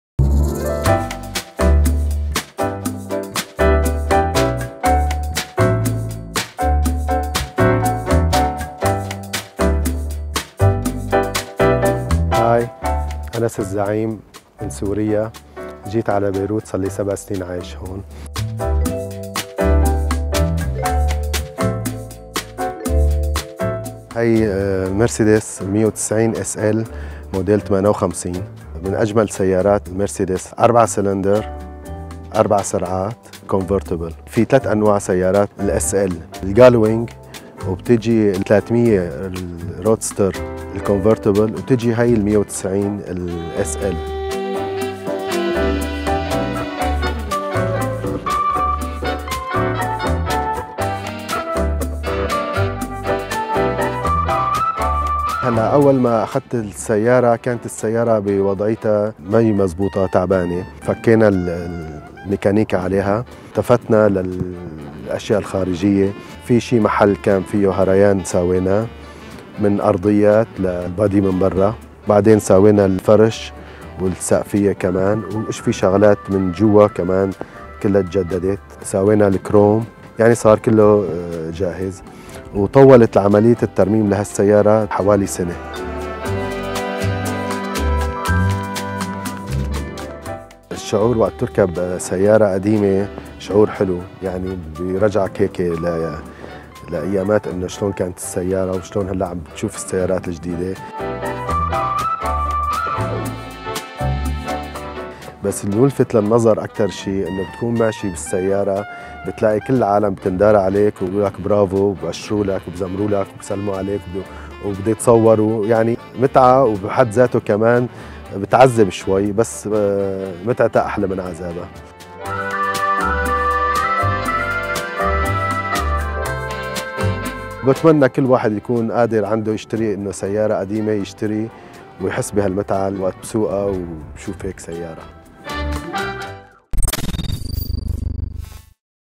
مرسيدس بنز 1958 الكلاسيكية: شغف لا حدود له - Classic Mercedes-Benz SL
مرسيدس بنز اس ال... سيارة كلاسيكية قديمة مغمورة بشغف صاحبها
قرر انس الزعيم اقتناء السيارات الكلاسيكية منذ ان كان جدّه يحب هذه السيارات، وبدأت هذه الهواية بالنمو مع الوقت حتى تحولت الى مهنته
وسيارة مرسيدس بنز اس ال 190  التي يقتنيها انس يصل سعرها الى حوالي
 200,000
دولار اميركي.
ما الذي دفع انس الزعيم باقتناء احدى اشهر سيارات مرسيدس بنز الرودستر في العالم؟ اليكم قصته مع سيارة مرسيدس بنز اس ال 190

Anas al Zaaim's Classic Mercedes Benz 190 SL is a V4 Roadster that took one year to be restored!

لا تنس الإشتراك معنا وفعّل الجرس لئلا يفوتك أي فيديو!